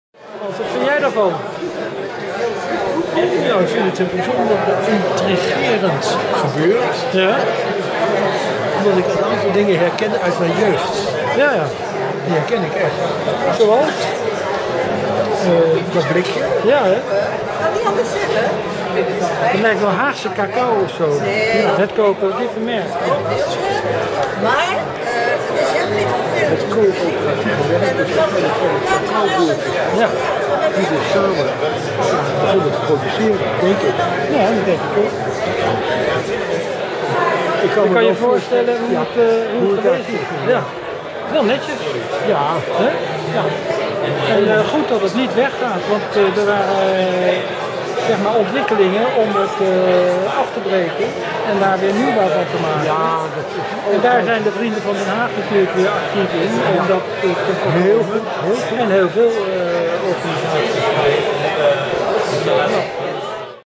United We Work bezoek
Samen werken als thema voor de toekomst, bezocht met mijn oudste broer Hans. Hij leidt yogaleraren op en is eigenaar van de Blikopener in Tilburg. Met hem is het leuk en verrassend deze expositie annex presentatie van samenwerkingsverbanden te bezoeken. Zie ook hagazine.nl